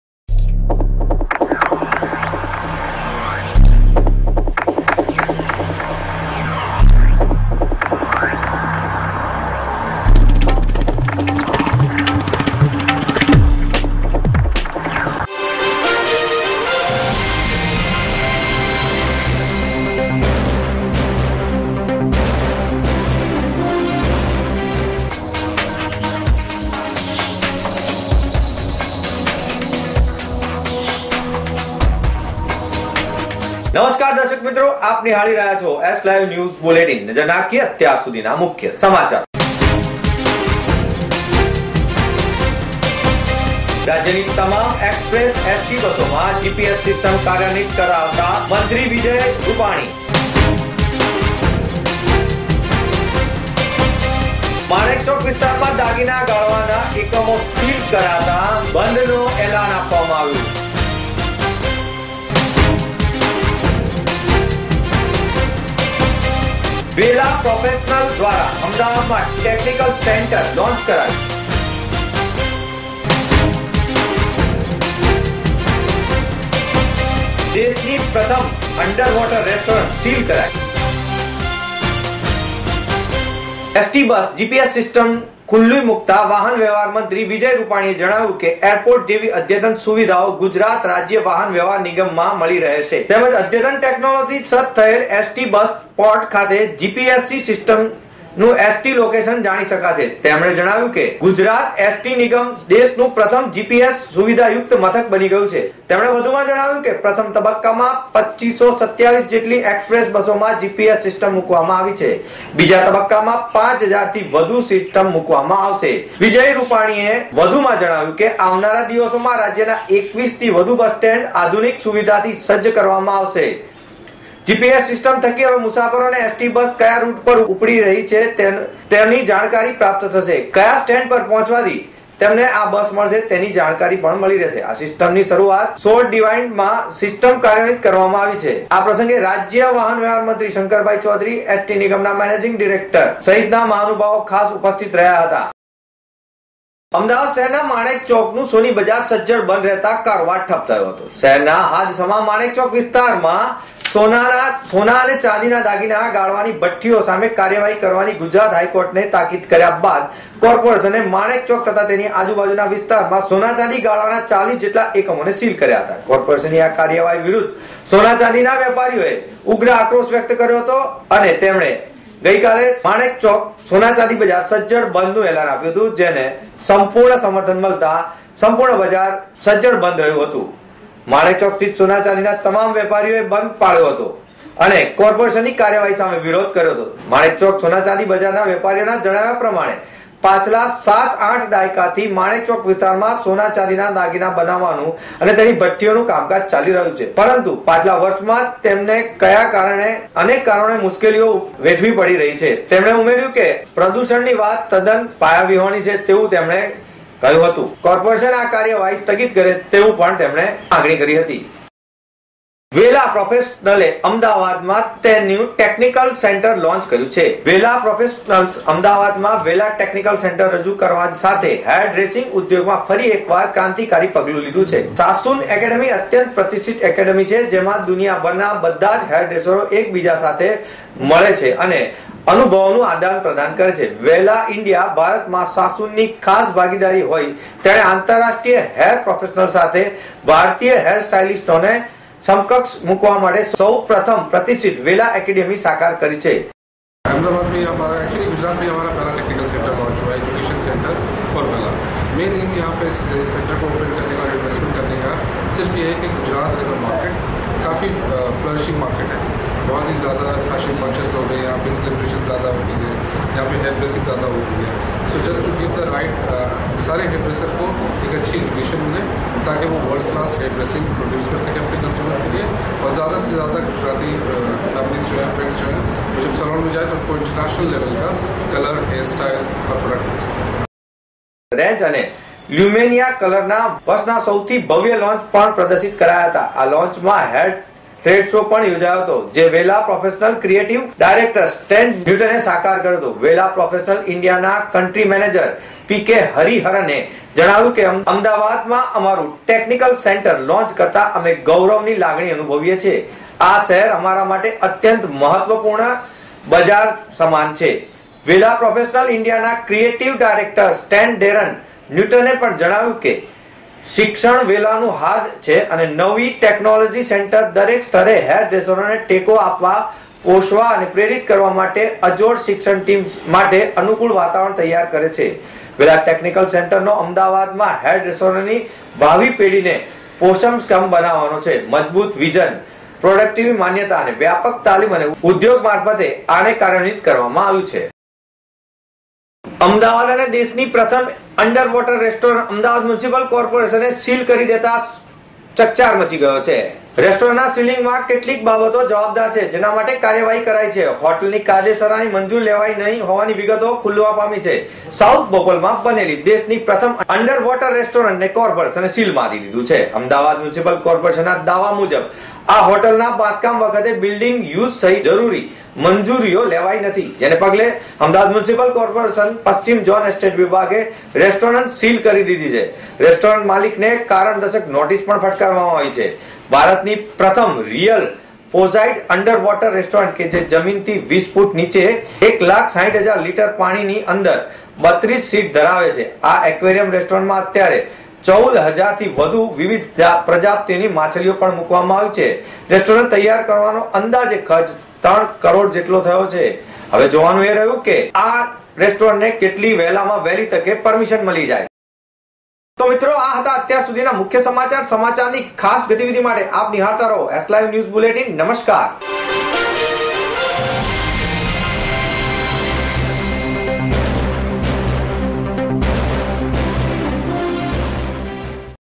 S Live News 05 02 2016
રાજ્યની તમામ એક્સપ્રેસ એસટી બસોમાં જીપીએસ સિસ્ટમ સુવિધા ઉપલબ્દ કરાવાશે – વિજયભાઈ રુપાણી
માણેકચોક વિસ્તારમાં દાગીના ગાળવાના એકમો સીલ કરાયા
વેલા પ્રોફેશનલ્સ દ્વારા અમદાવાદમાં ટેકનીકલ સેન્ટર લોન્ચ કરાયું
દેશની પ્રથમ અંડર વોટર રેસ્ટોરન્ટ અંતે સીલ થઈ